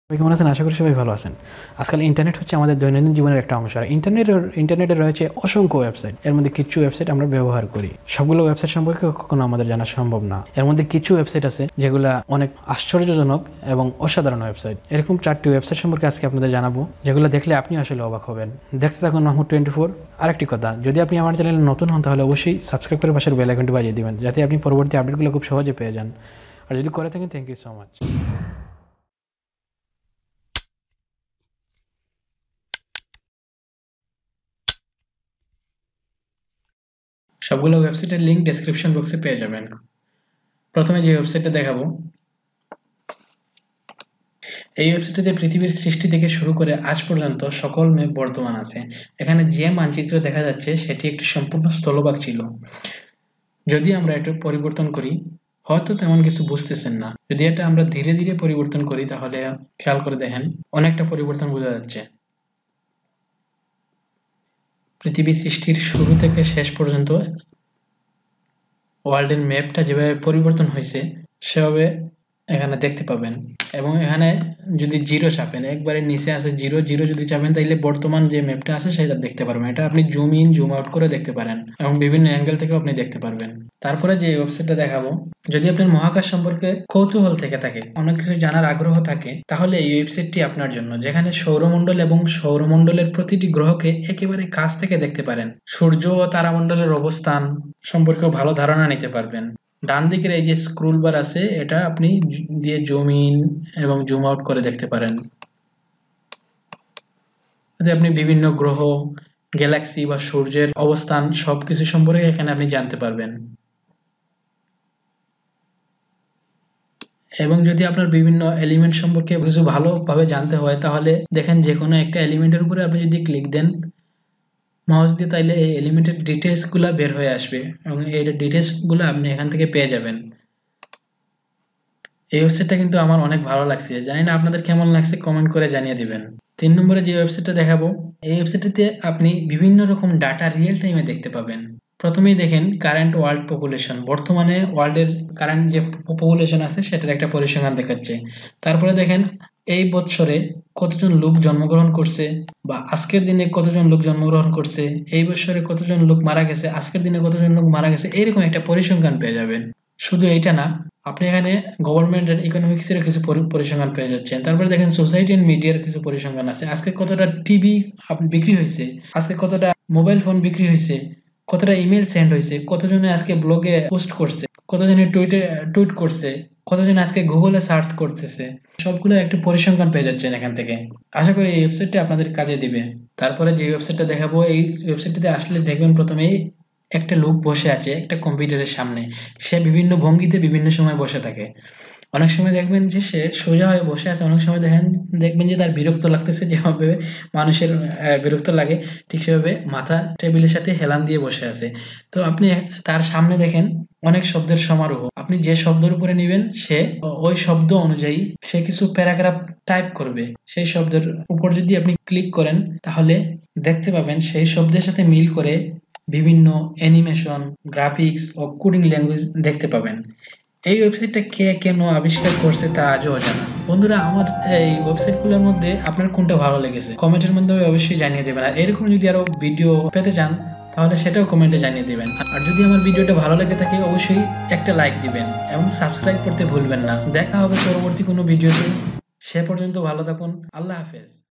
4 amazing website in the world | mahmud 24
বিশ্বের ৪ টি রহস্যময় অজানা ওয়েবসাইট | 4 amazing website in the world
This video shows you, The Most Amazing Cool Website You Didn't Know Existed On Internet!